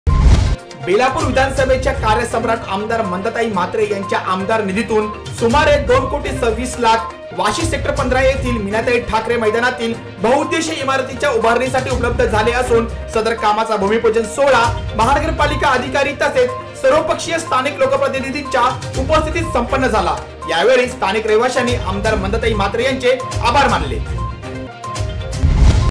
वाशी_आ. मंदाताई म्हात्रे यांच्या आमदार निधीतील ‘बहुउद्देशीय ईमारत उभारणीच्या कामाचे भूमिपूजन’..!
'Bhoomipujan of multi-purpose building construction work' funded by Mla Mandatai Mhatre at Vashi..!